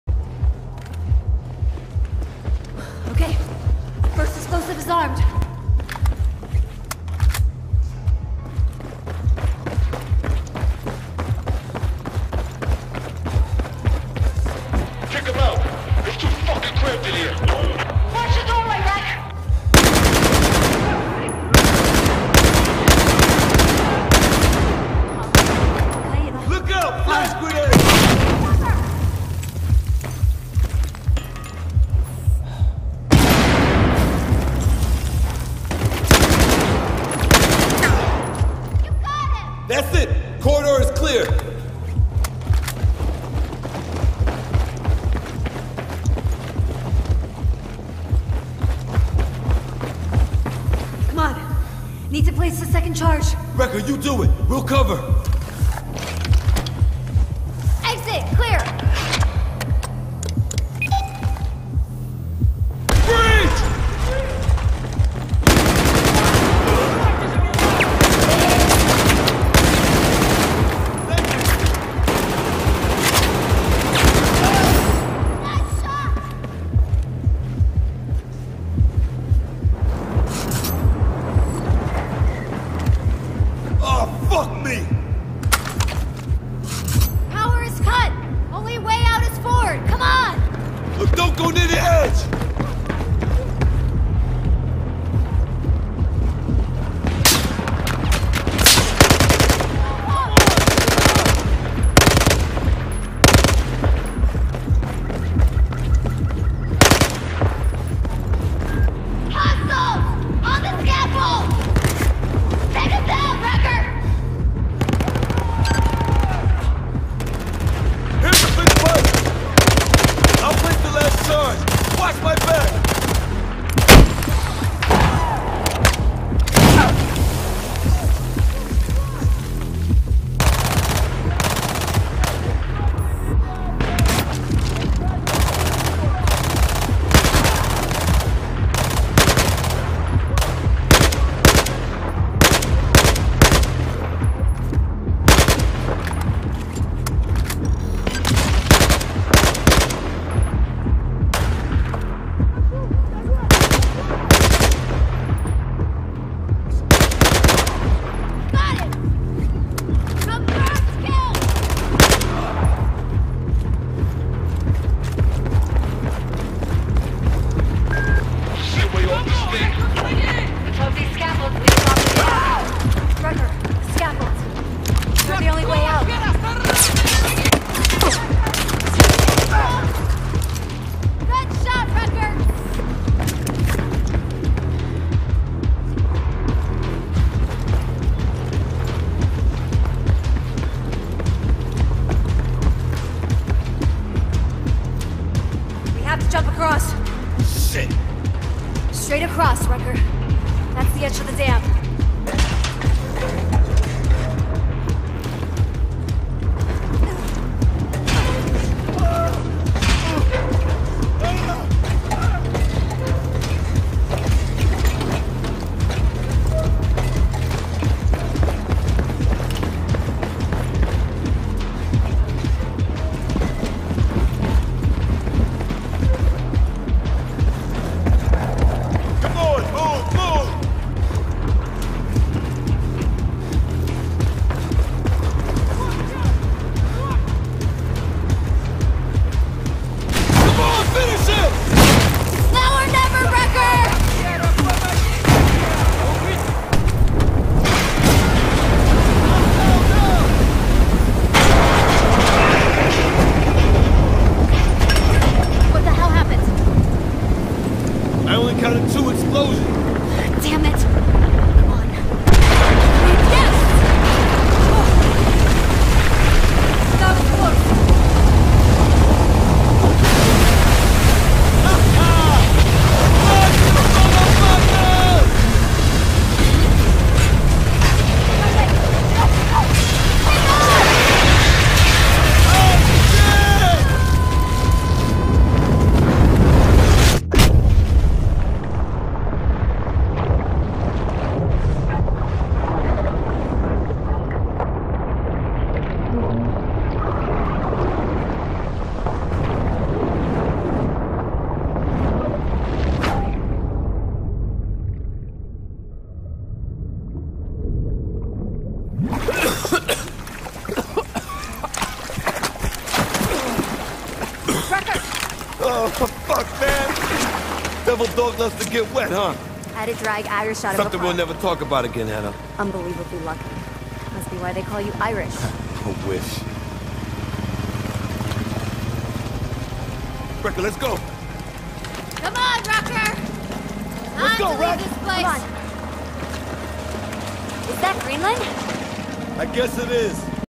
Blowing up the Dam!
Fun part to this game. A friendly base is being surrounded and blowing up this Dam is to flood the enemy. Freeing our team.